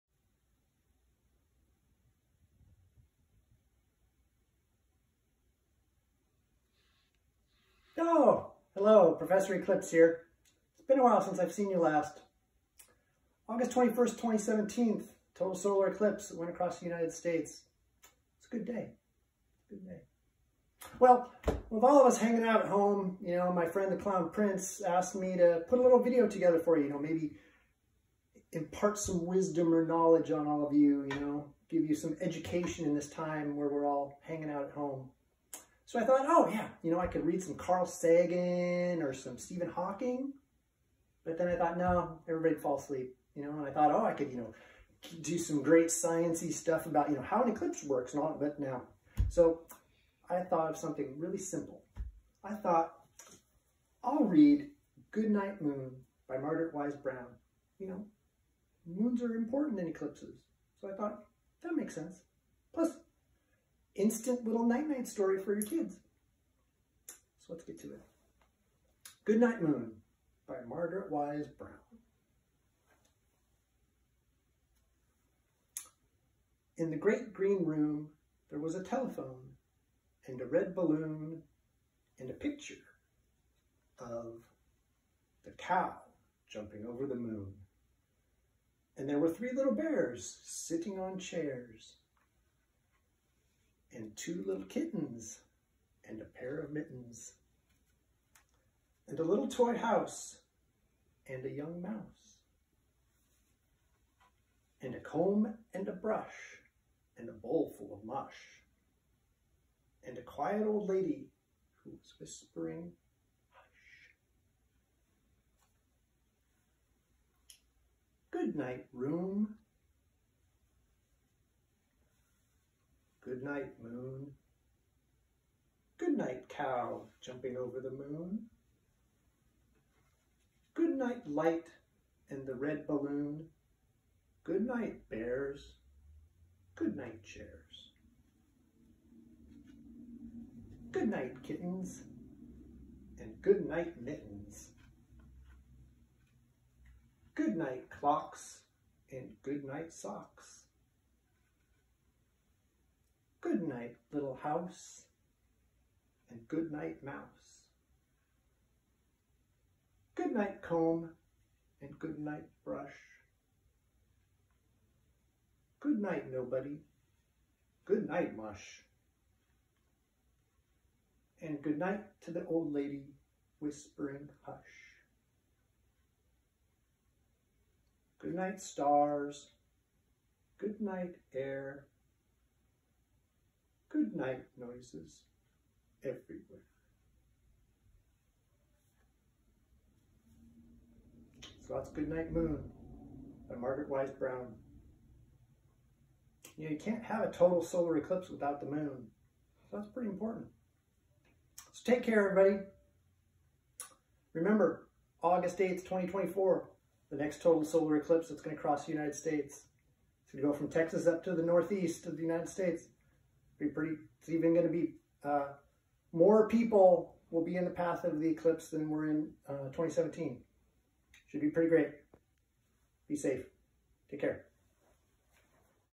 Prof E. Clipse Goodnight Moon
Rose Festival 'scientist' Professor E. Clipse shares some of his knowledge with a reading to keep people informed and entertained during these crazy times!